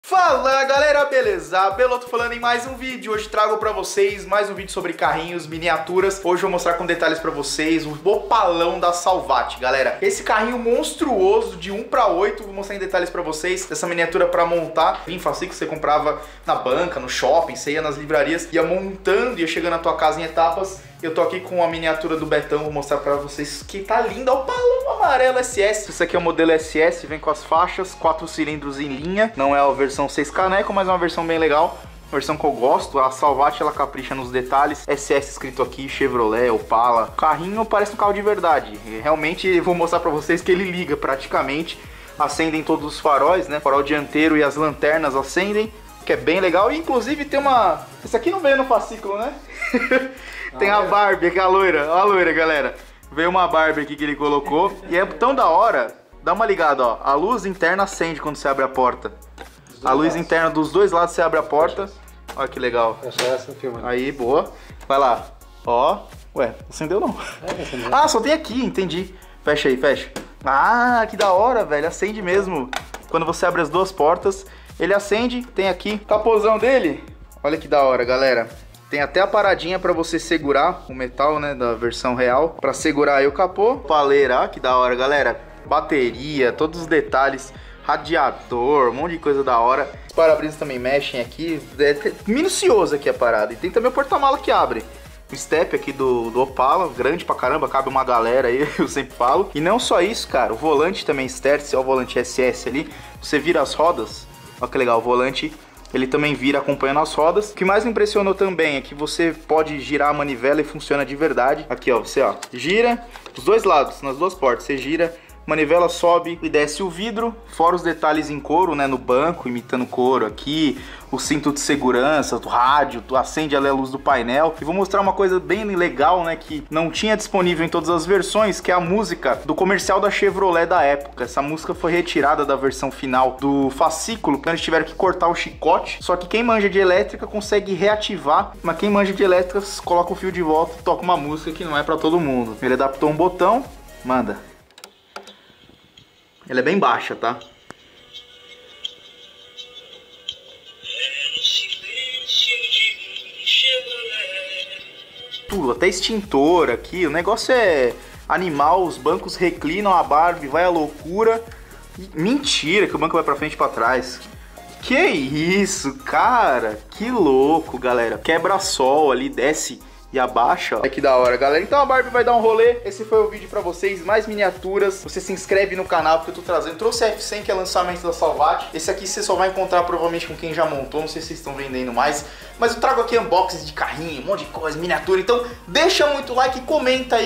CHEVROLET OPALA EM MINIATURA 1:8 QUE FUNCIONA DE VERDADE!
Chevrolet Opala escala 1/8 da Editora Salvat montado completo com diversas funções!